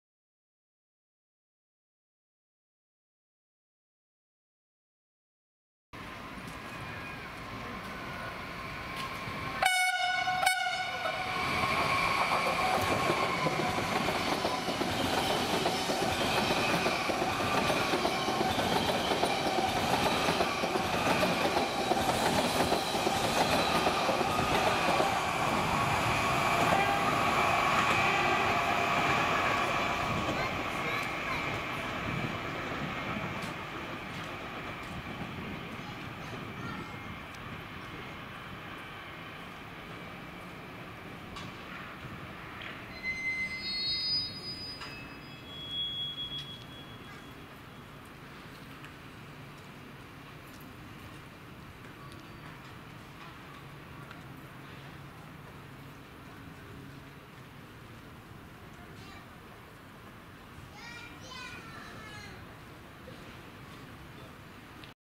ЭВС1-13 "Сапсан" на платформе Фарфоровская | (Siemens Velaro RUS В1) | EVS1-13
ЭВС1-13 "Сапсан" (Siemens Velaro RUS В1) проходит платформу Фарфоровская
г. Санкт-Петербург,
Октябрьская железная дорога.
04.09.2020

EVS1-13 (Siemens Velaro RUS В1)
Farforovskaya platform
St.Petersburg, Russia.